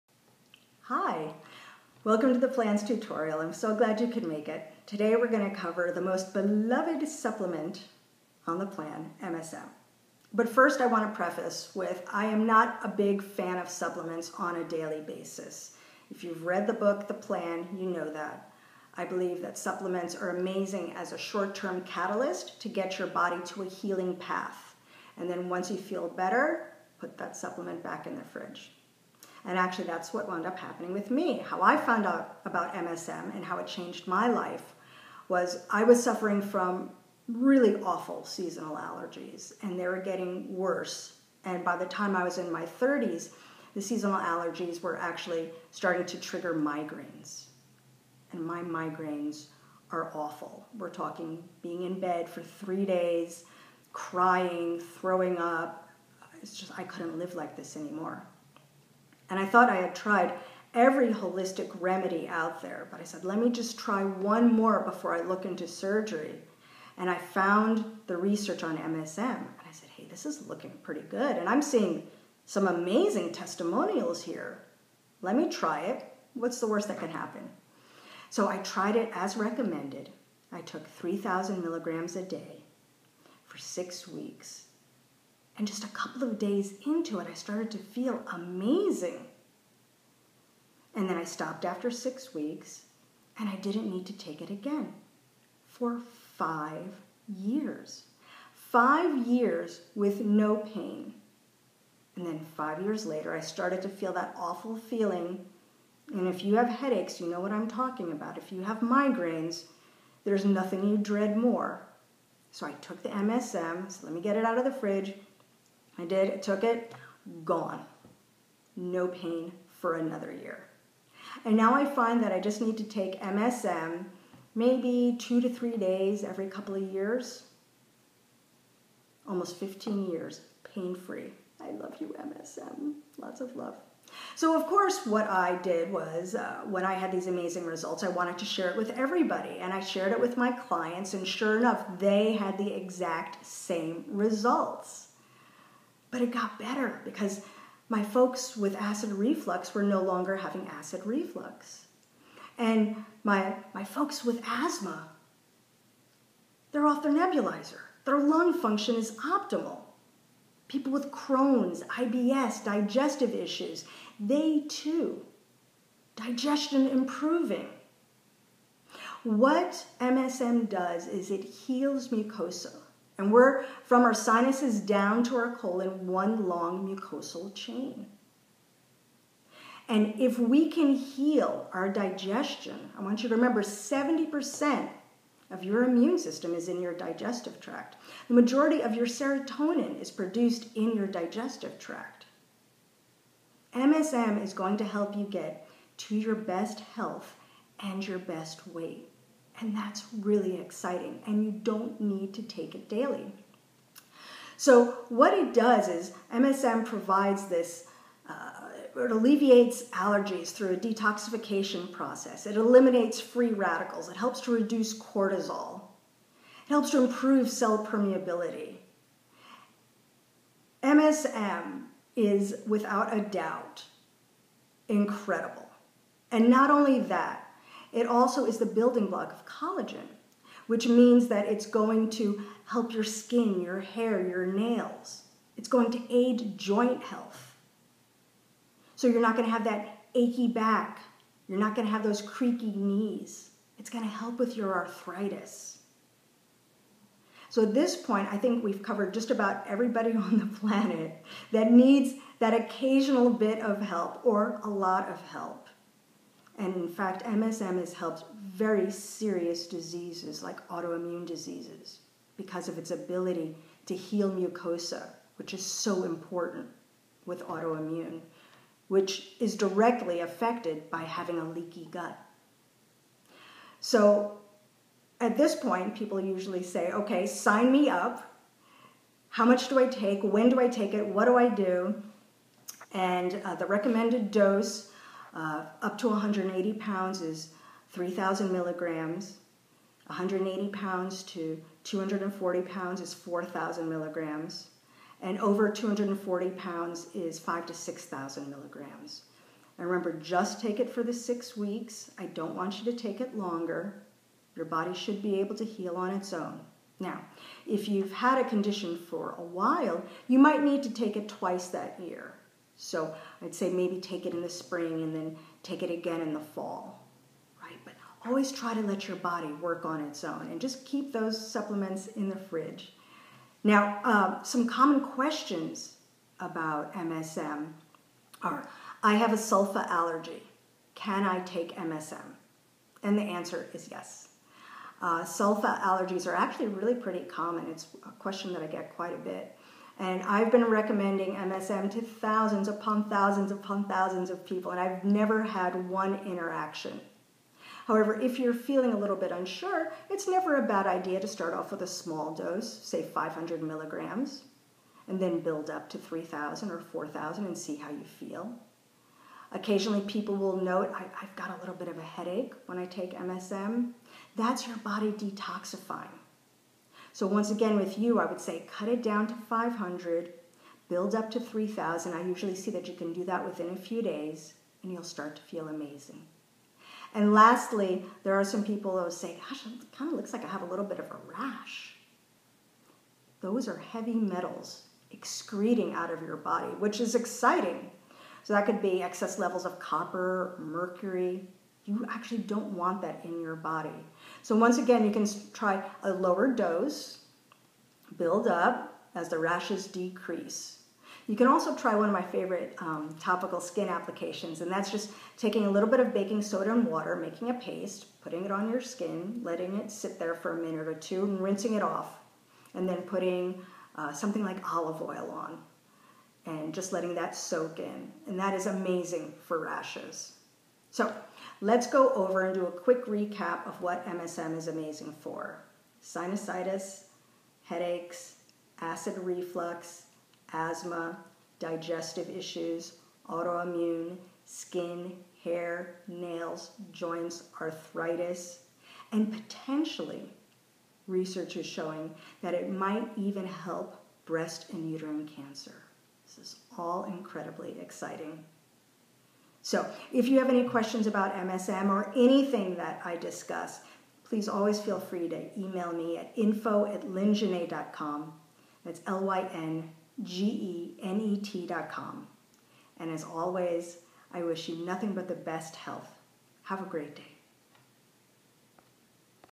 Msm
MSM helps to alleviate the symptoms of a large number of allergies including food allergies, contact allergies, inhalation allergies, etc. The major anti-allergic property of MSM is caused by its ability to bind to the mucosa and present a natural blocking interface between hosts and allergens.